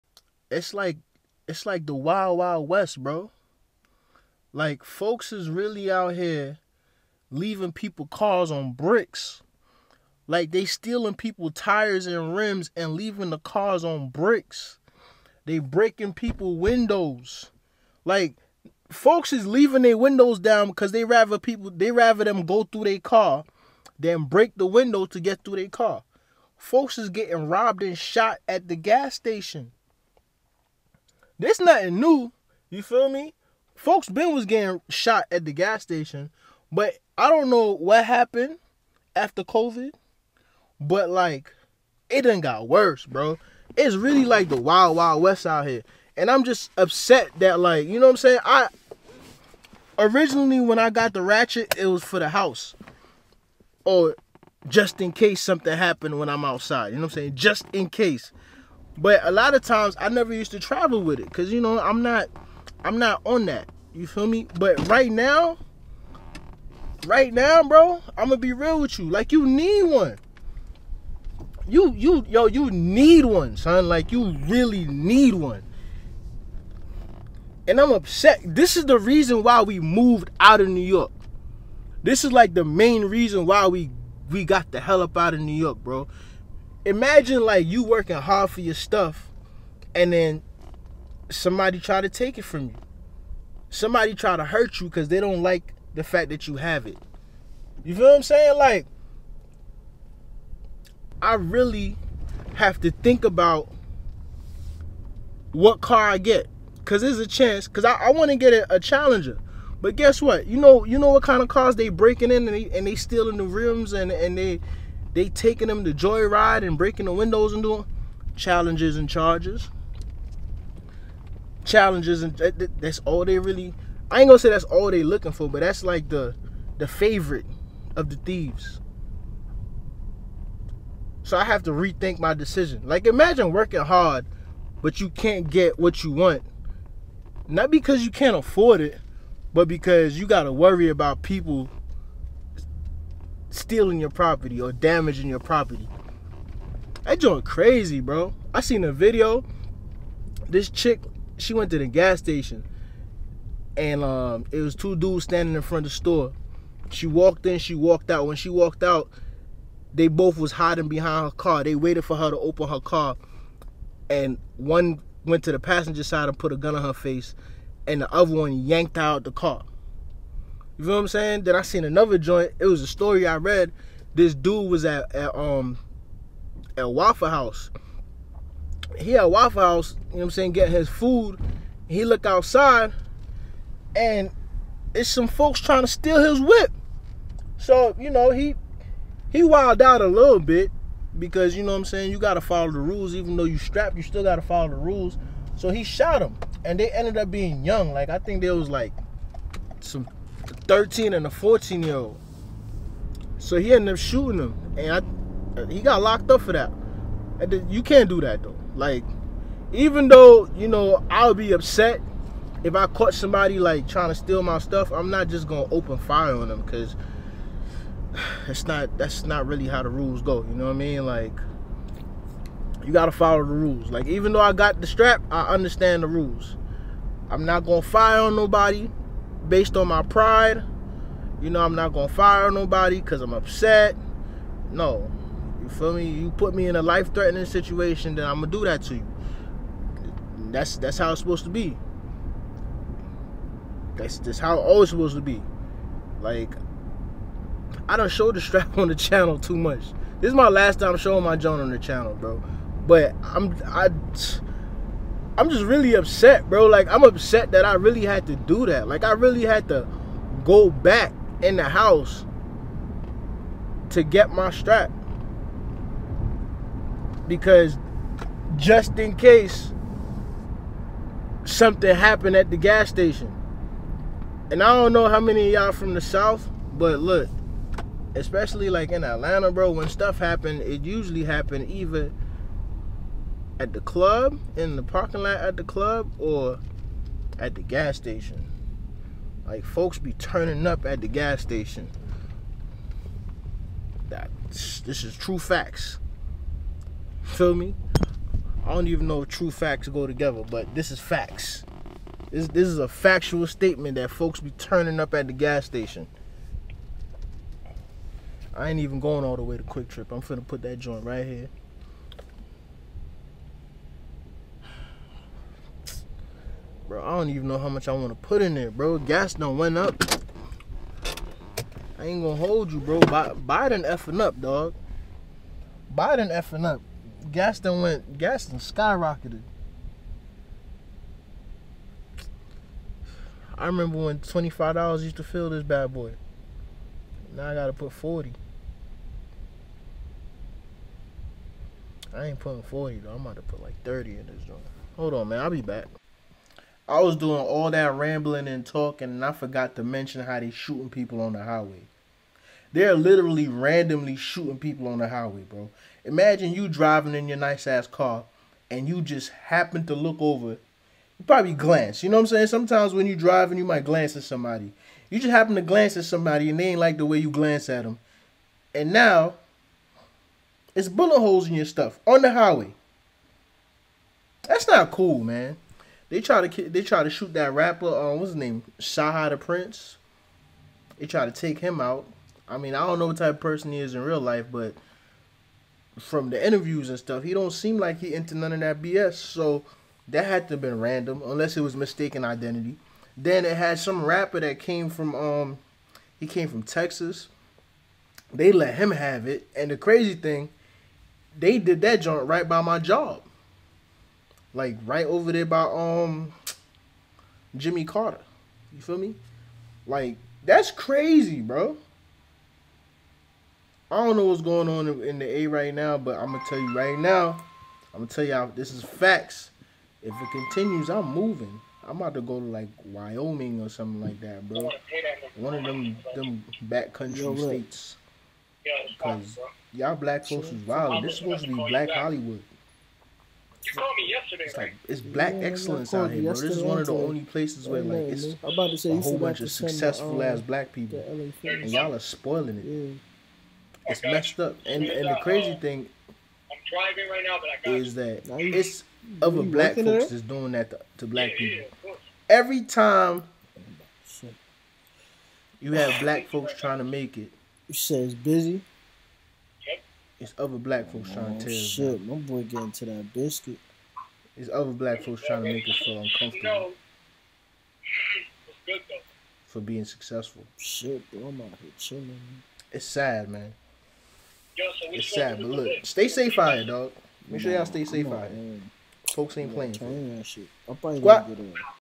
My Life In Atlanta Update They Left My Car Sitting On Bricks
Join The Sqaudd